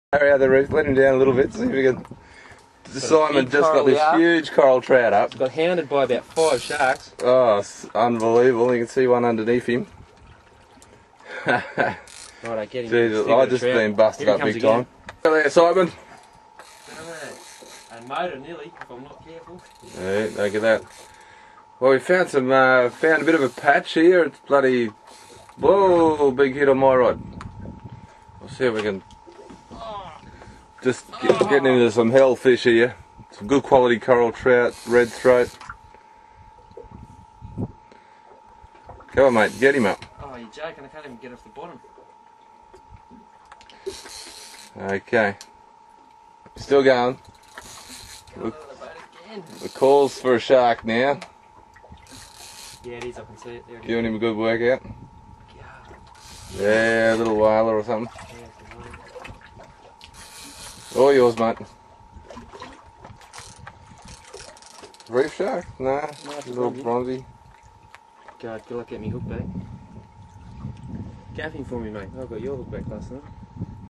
Whaler Sharks Give a good fight!! Top Fishing!!
Bronze whaler sharks can be a pest at times on the reef, but they definately give you a good workout.  Especially when they take your quality reef fish when fishing.  Sometimes they get to thick and you have no choice but to move fishing spots.